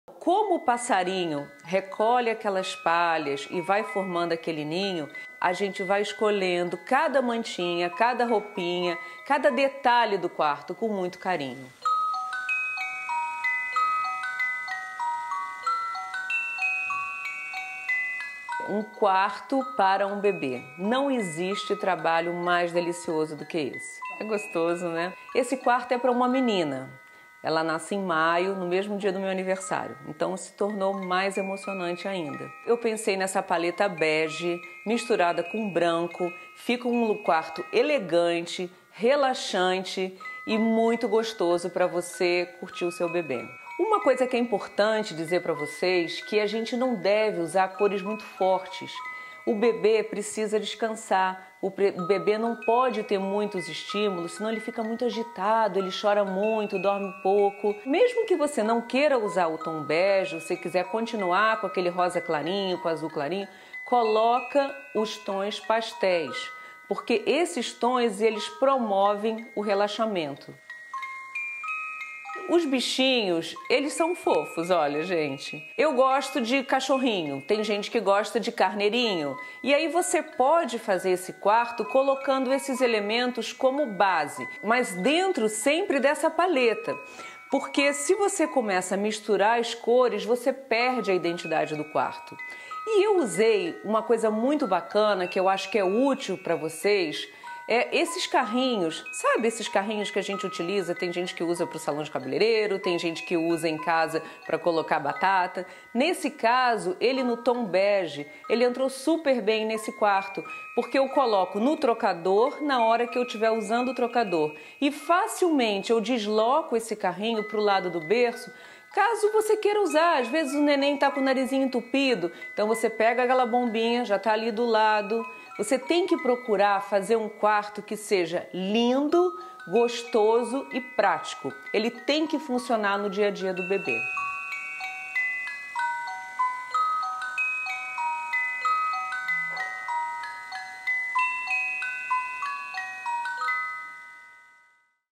IDÉIAS DE DECORAÇÃO PARA O QUARTO DO BEBE 1 | LU MARINHO
Aprenda a arrumar um quarto de bebê delicado, calmo e acolhedor usando cores neutras, detalhes divertidos e tendo tudo o que você precisa super a mão !!
Beleza e funcionalidade by Lu Marinho !